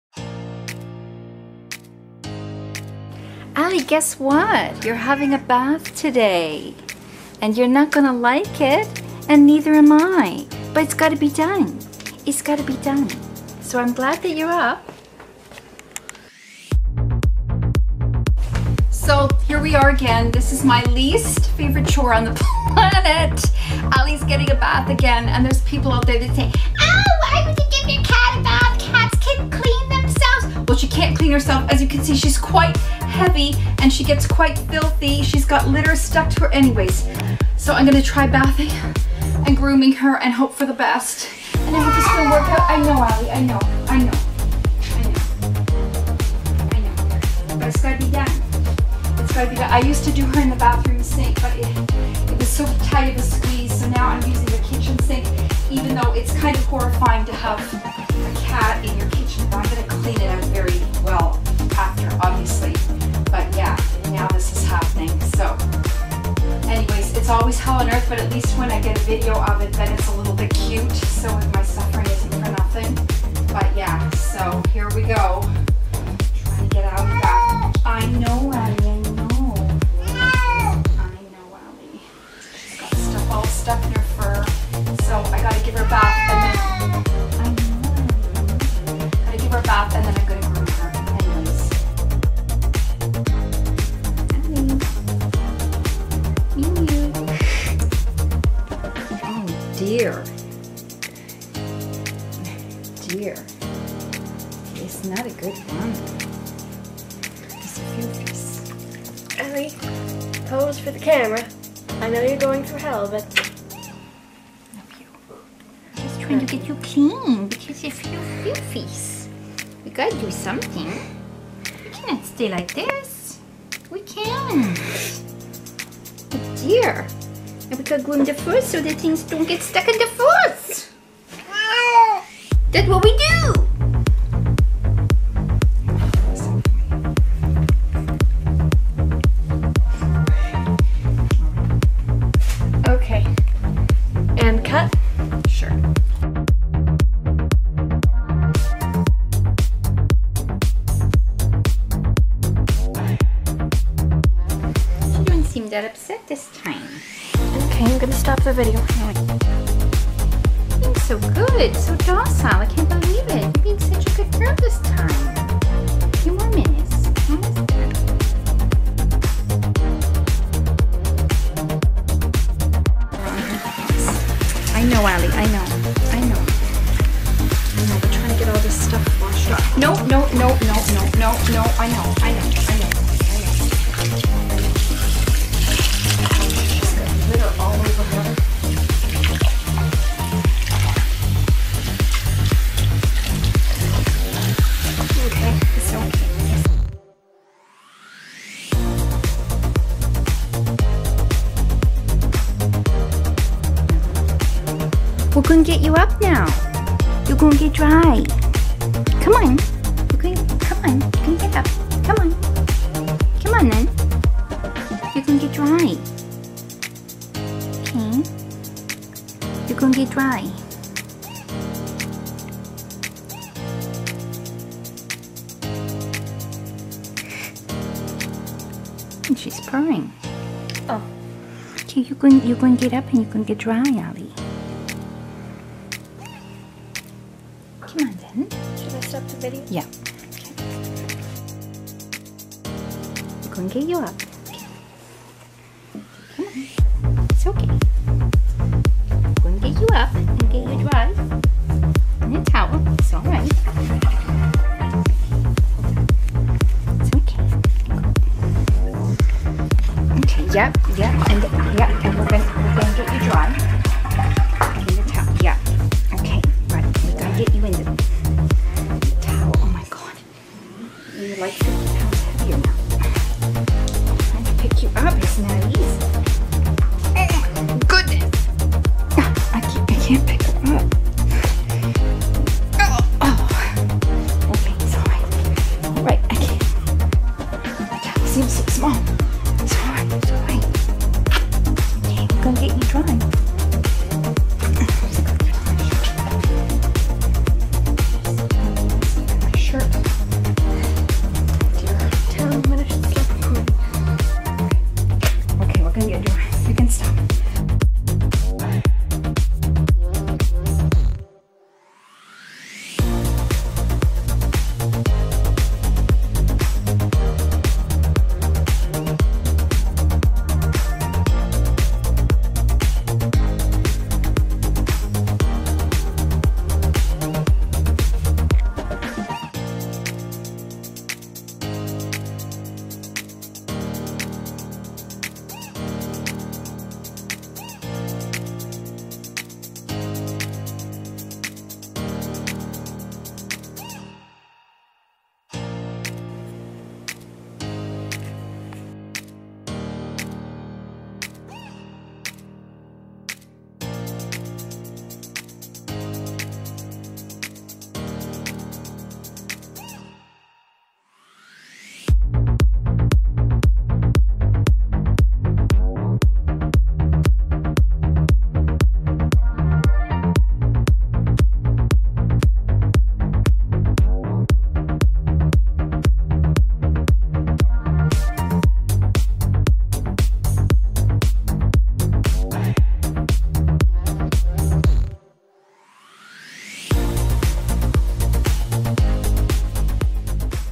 Bathing Ali AGAIN!
Happy Caturday! Last Caturday was not so happy. I had to give Ali a bath AGAIN! As you may have seen in other videos, Ali is a BIG cat & can't reach her butt to clean herself properly so I have to bathe her & trim her fur. Unfortunately it doesn't take long for her to get filthy again. The bath is always hell for Ali and for me but at least when I get cute videos & pictures of her then it's not a total loss! This time Ali seemed more calm/relaxed than usual (she may have just been in FREEZE mode?) Anyway I hope you enjoy this. Michelle shot some of the video & Ali had some great expressions!